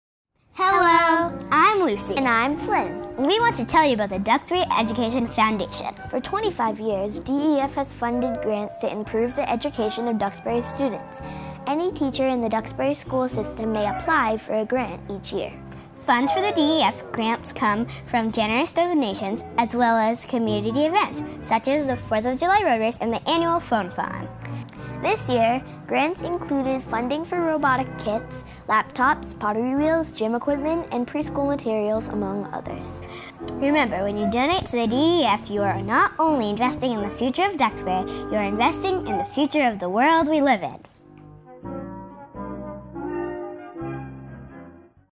Duxbury Education Foundation
The Duxbury Education Foundation provides grants to teachers and schools within the Duxbury School system.